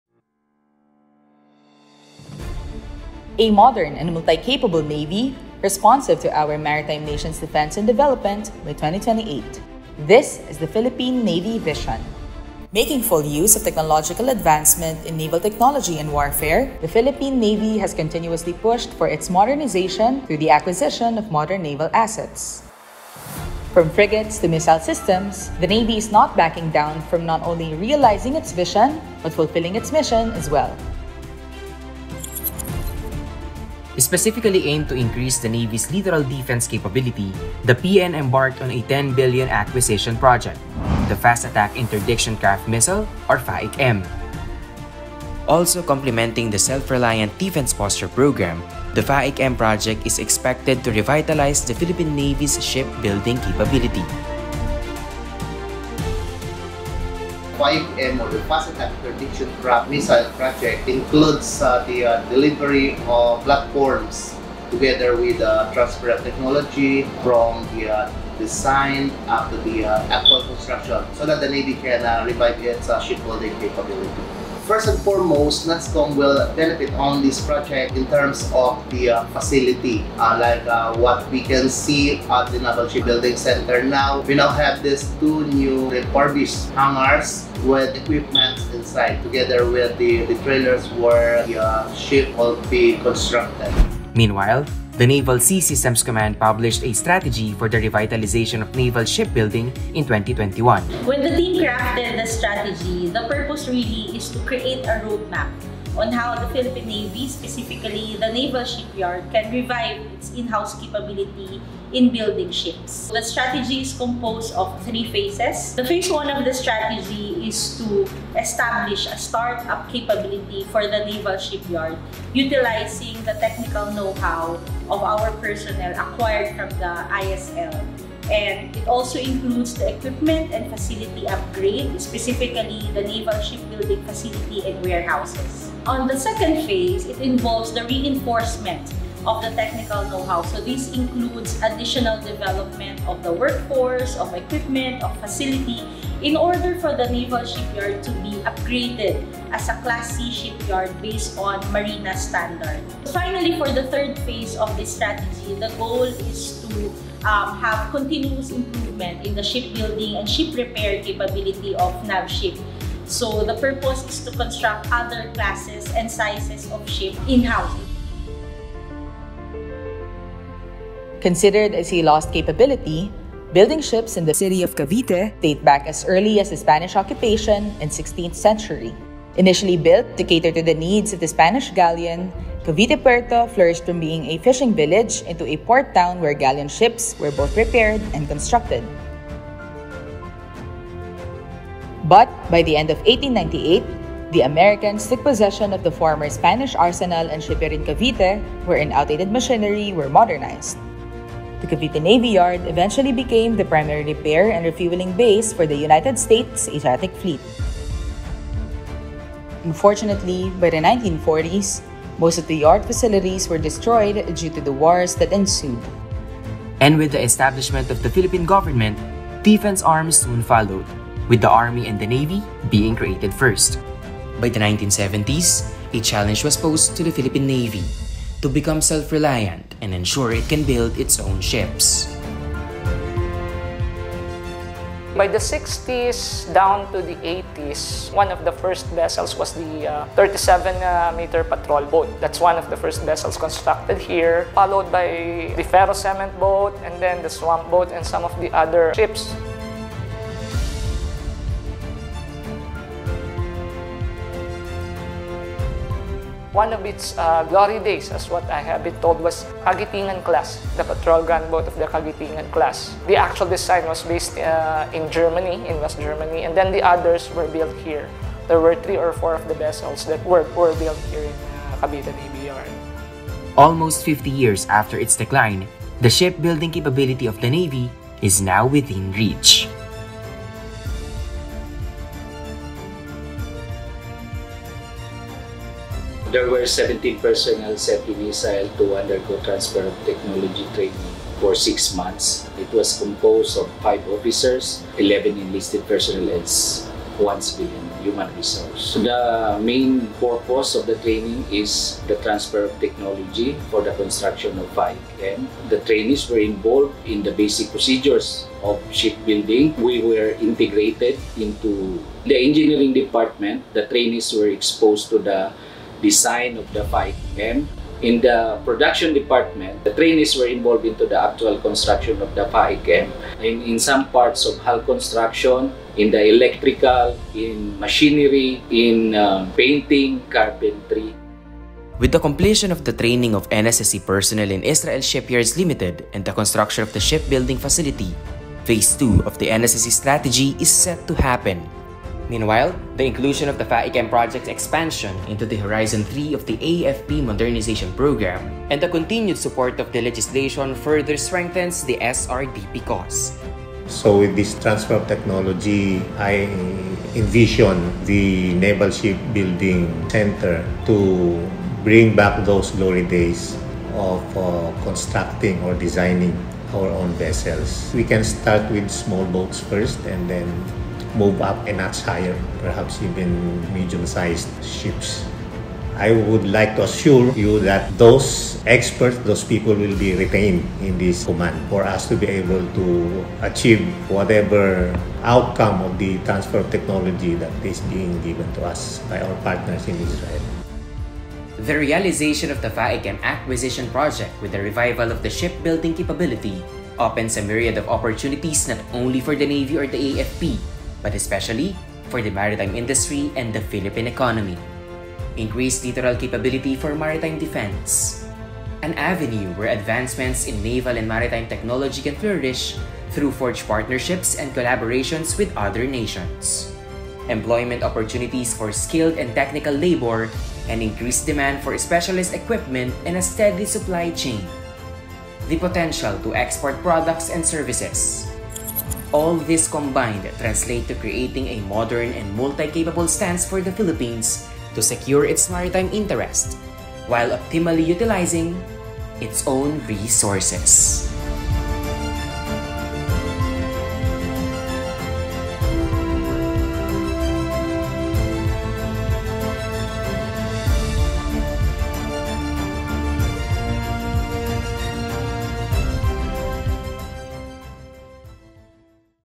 The Philippine Navy Shipbuilding Capability
The newly-refurbished Naval Shipbuilding Center of the Naval Sea Systems Command was unveiled, along with the christening of the two (2) new additional Shaldag Mk V units of the Philippine Navy.